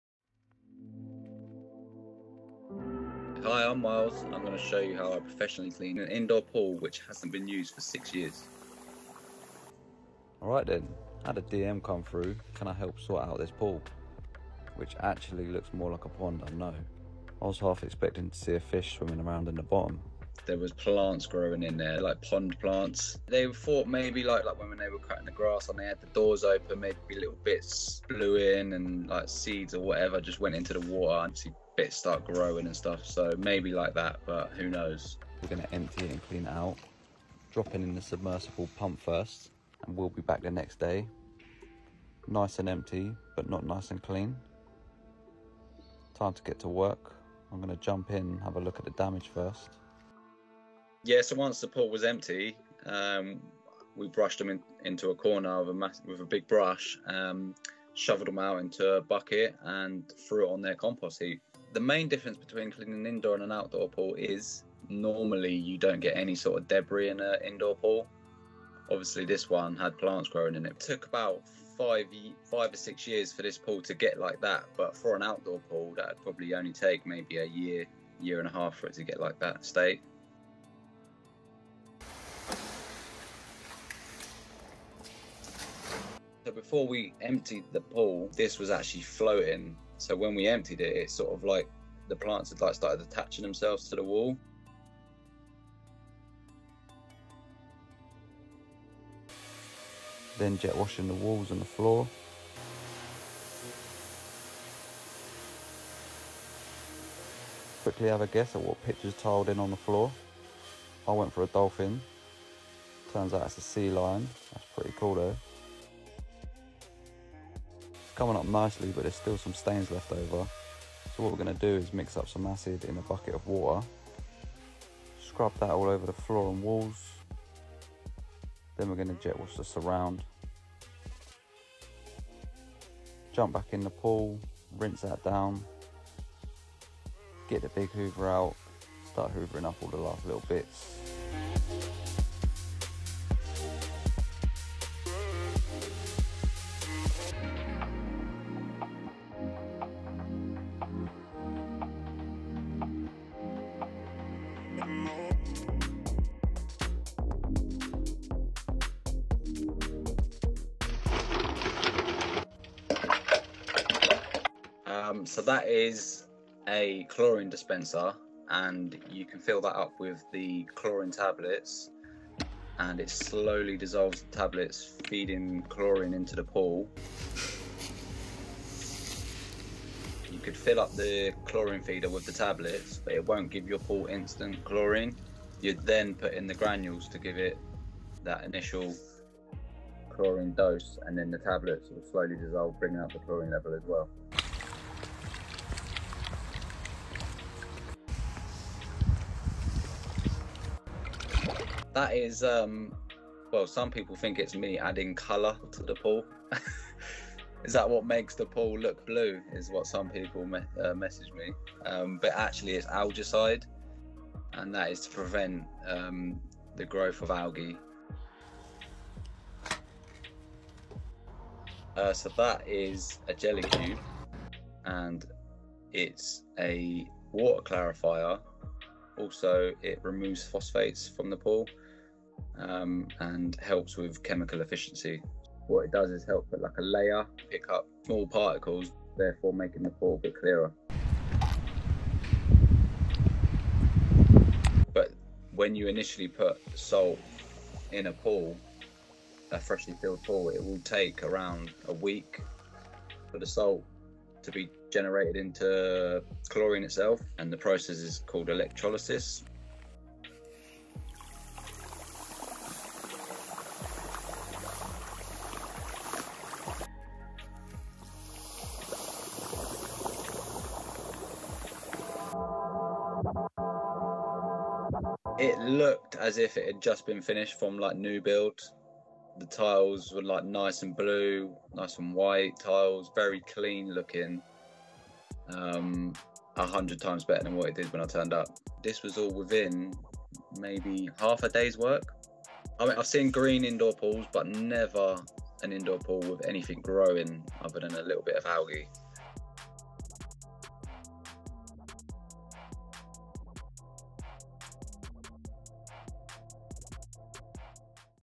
How An Abandoned Indoor Pool Is Deep Cleaned | Insider
Miles Laflin (@thep00lguyml) deep cleans the dirtiest pools in the UK.  In this episode, he takes on an abandoned pool that hasn’t been used or cleaned in five to six years and is filled with pond plants and algae. Miles takes us through the process of deep cleaning step by step, showing us the transformation from unusable pool to making it look brand new.